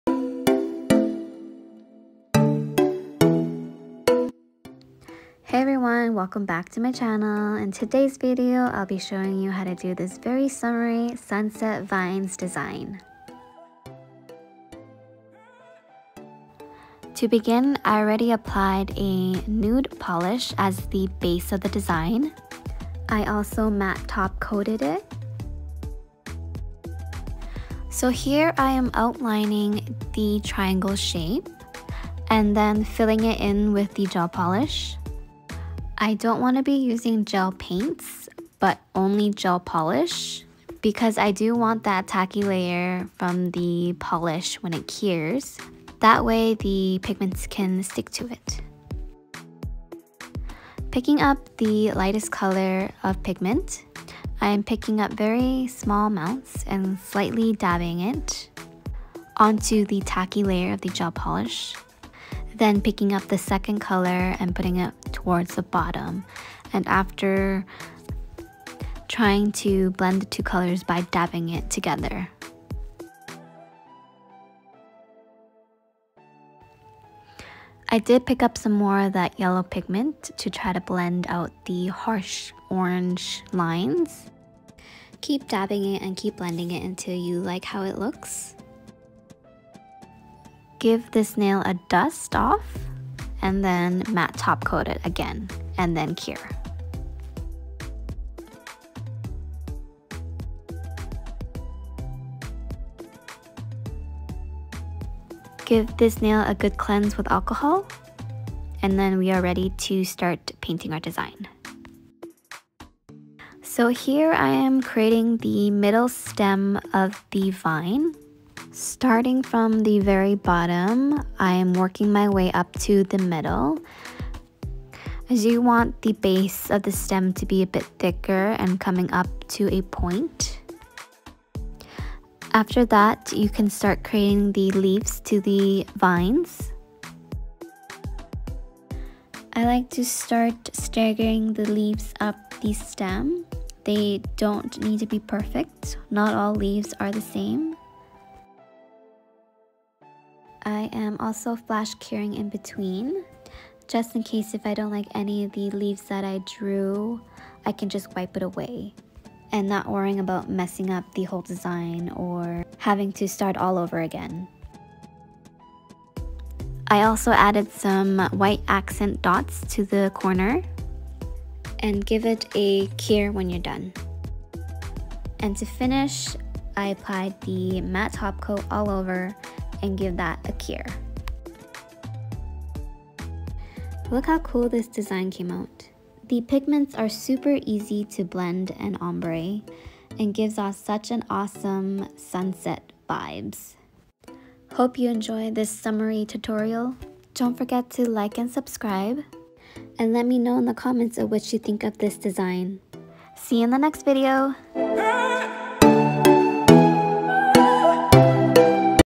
Neon Vines Nail Art Tutorial
Chasing summer with this neon and nude set! I’m loving the look of mixing contrasting colors and  dolling up nudes! Line work can sometimes be challenging when hand painting straight lines so switch it up and try curved lines. This may help you build up a steady hand and confidence with freehand painting. Can’t wait to see your neon vines inspired nail art!